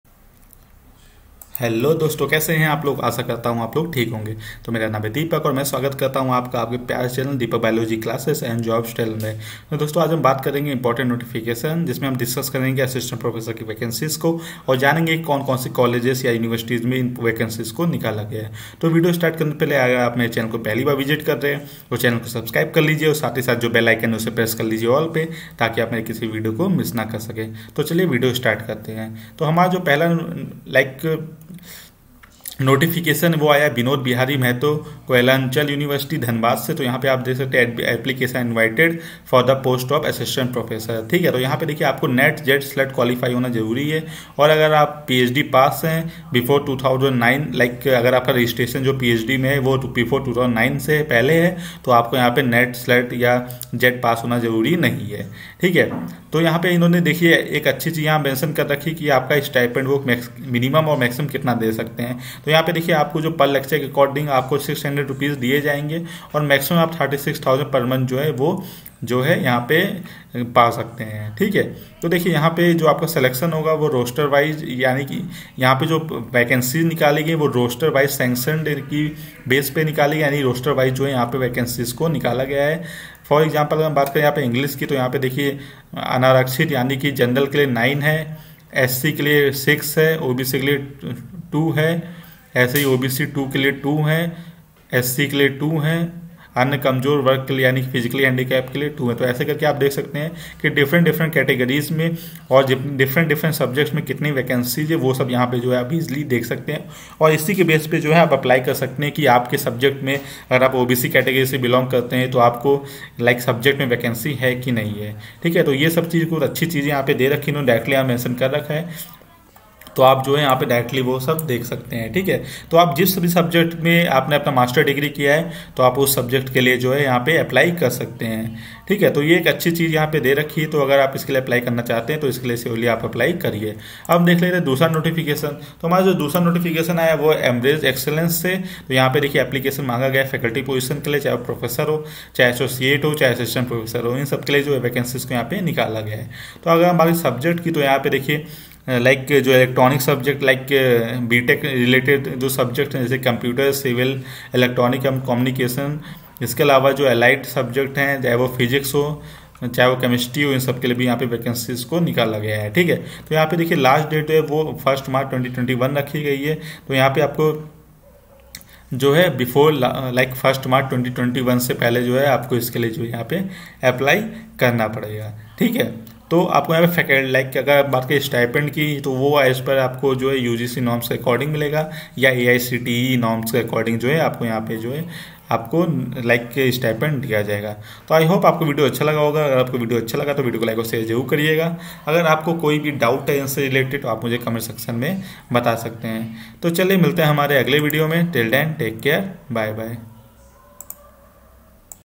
Multiple Assistant Professor vacancies in all subjects II Teaching positions
In this video tutorial, we have discussed about the various Assistant Professor vacancies mostly in all subjects. so if you are eligible for the post then apply it..